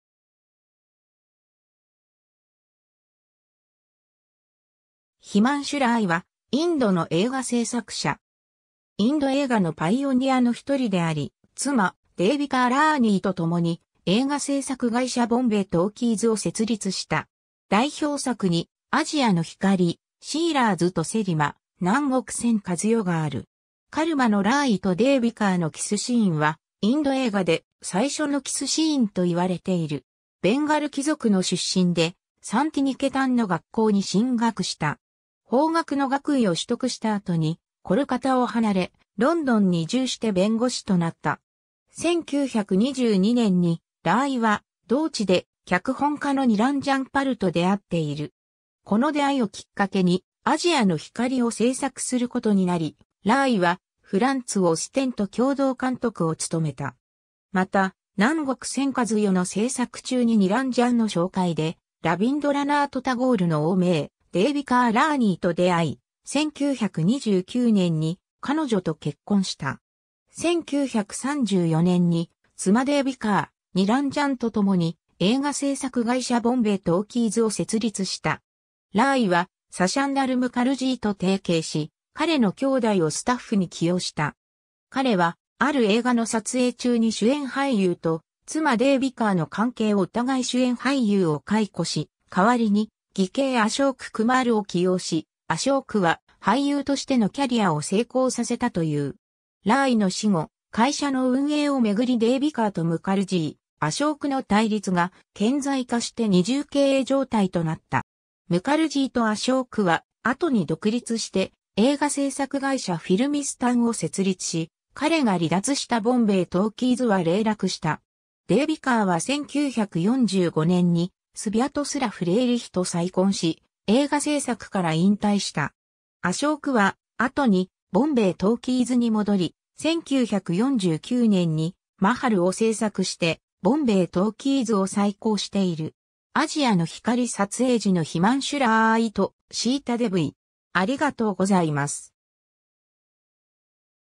ヒマンシュ・ラーイ
ヒマンシュ・ラーイ（Himanshu Rai、1892年 - 1940年5月16日）は、インドの映画製作者。
インド映画のパイオニアの一人であり、妻デーヴィカー・ラーニーと共に映画製作会社ボンベイ・トーキーズを設立した。
代表作に『亜細亜の光』『シーラーズとセリマ』『南国千一夜』がある。
 『カルマ』のラーイとデーヴィカーのキスシーンは「インド映画で最初のキスシーン」と言われている。
 ベンガル貴族の出身で、サンティニケタンの学校に進学した。
法学の学位を取得した後にコルカタを離れ、ロンドンに移住して弁護士となった。
1922年にラーイは同地で脚本家のニランジャン・パルと出会っている。
この出会いをきっかけに『亜細亜の光』を製作することになり、ラーイはフランツ・オステンと共同監督を務めた。
また、『南国千一夜』の製作中にニランジャンの紹介でラビンドラナート・タゴールの大姪デーヴィカー・ラーニーと出会い、1929年に彼女と結婚した。
 1934年に妻デーヴィカー、ニランジャンと共に映画製作会社ボンベイ・トーキーズを設立した。
ラーイはサシャンダル・ムカルジーと提携し、彼の兄弟をスタッフに起用した。
彼は、ある映画の撮影中に主演俳優と妻デーヴィカーの関係を疑い主演俳優を解雇し、代わりに義兄アショーク・クマールを起用し、アショークは俳優としてのキャリアを成功させたという。
 ラーイの死後、会社の運営を巡りデーヴィカーとムカルジー、アショークの対立が顕在化して二重経営状態となった。
ムカルジーとアショークは後に独立して映画製作会社フィルミスタンを設立し、彼が離脱したボンベイ・トーキーズは零落した。
デーヴィカーは1945年にスヴャトスラフ・レーリヒと再婚し、映画製作から引退した。
アショークは後にボンベイ・トーキーズに戻り、1949年に『Mahal』を製作してボンベイ・トーキーズを再興している。
 『亜細亜の光』撮影時のヒマンシュ・ラーイとシータ・デヴィ